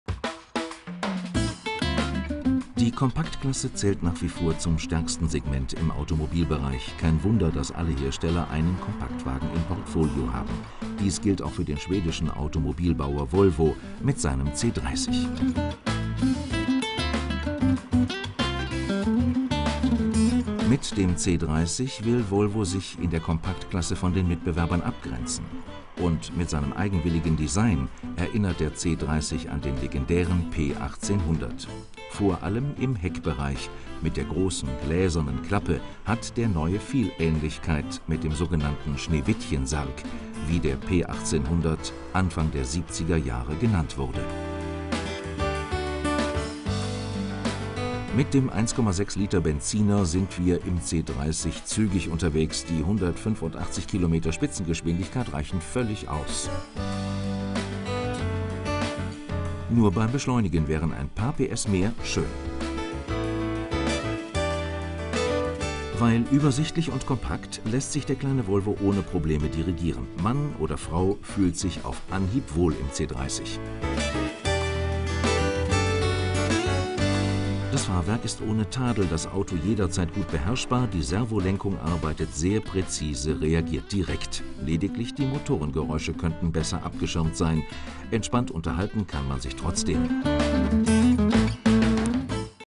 Volvo C30
Die Kompaktklasse zählt nach wir vor zum stärksten Segment im Automobilbereich. Kein Wunder, dass alle Hersteller ihren Kompaktwagen im Portfolio haben. Dies gilt auch für den schwedischen Automobilbauer Volvo, der hier seinen C 30 an den Start schickt.